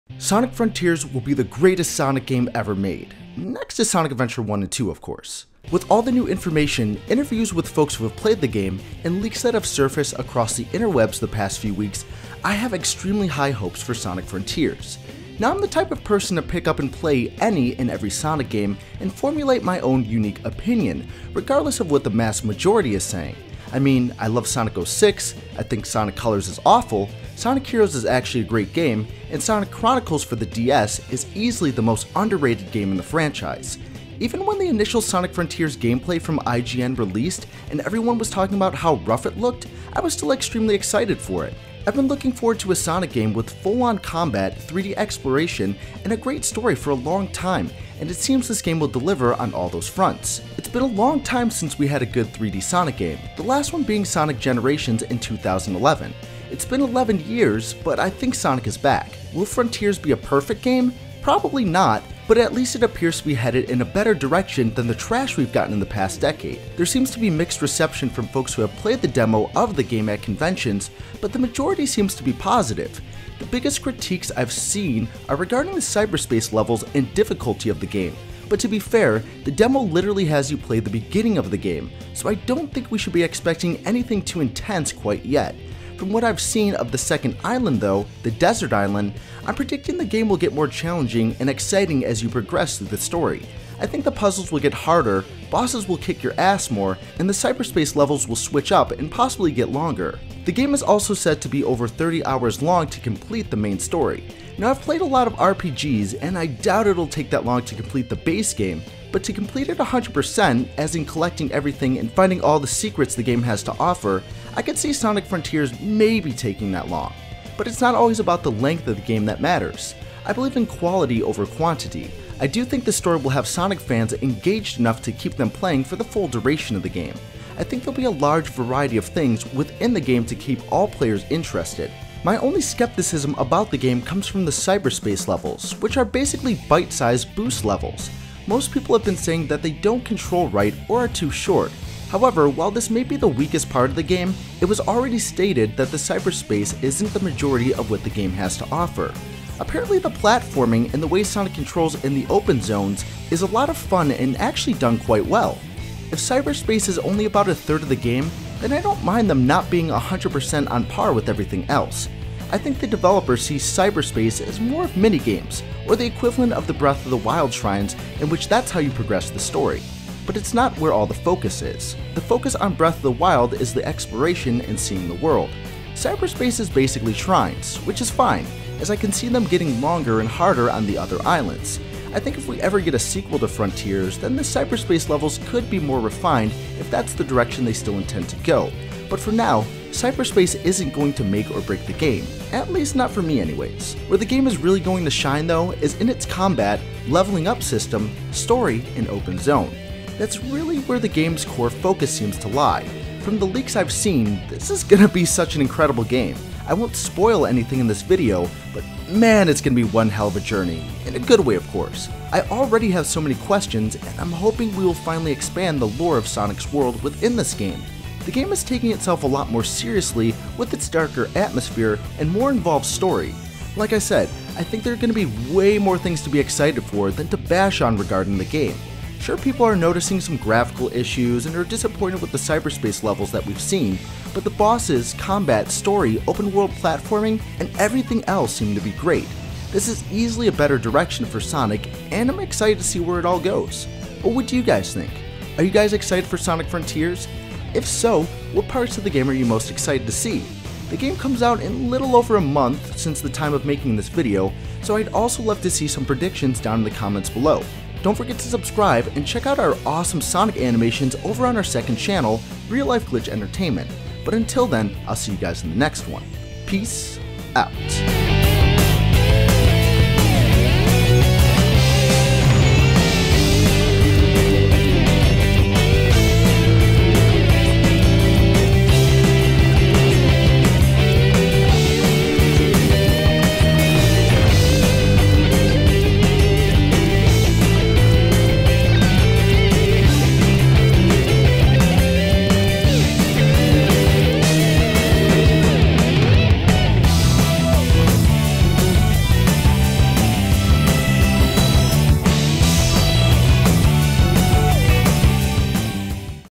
SONIC FRONTIERS is going to be THE GOAT and why you SHOULDN'T WORRY
Sonic Frontiers is going to be one of the greatest Sonic games of all time.

Our LIVE ACTION ANIME-INSPIRED SERIES!